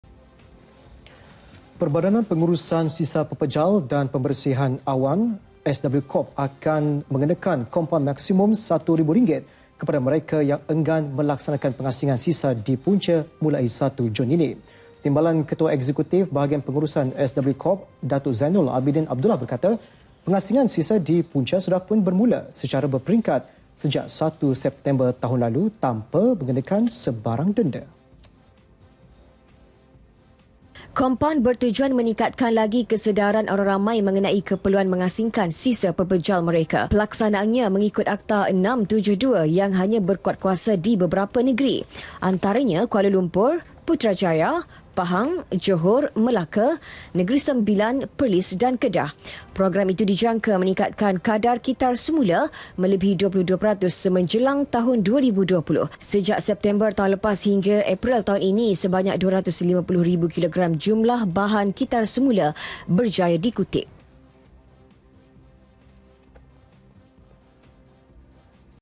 SISA DI PUNCA- DENDA RM1000 JIKA GAGAL BUAT PENGASINGAN [14 MEI 2016]
Perbadanan Pengurusan Sisa Pepejal  dan Pembersihan Awan ( SWcorp) akan mengenakan  kompaun maksimum  satu ribu ringgit,  kepada mereka  yang enggan melaksanakan  pengasingan sisa di punca, mulai 1 Jun ini.

Timbalan  Ketua Eksekutif Bahagian Pengurusan  SWcorp , Datuk Zainul Abidin Abdullah berkata,  pengasingan sisa di punca sudahpun bermula secara berperingkat sejak 1 September tahun lalu, tanpa mengenakan sebarang denda.